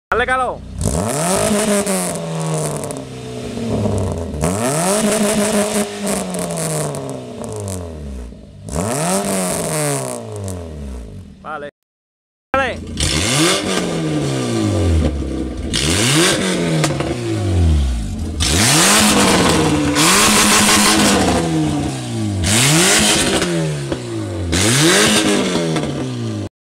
Citroen ZX 1.4i 8v - Escape libre
ya que se desmonta la línea, se prueba así xD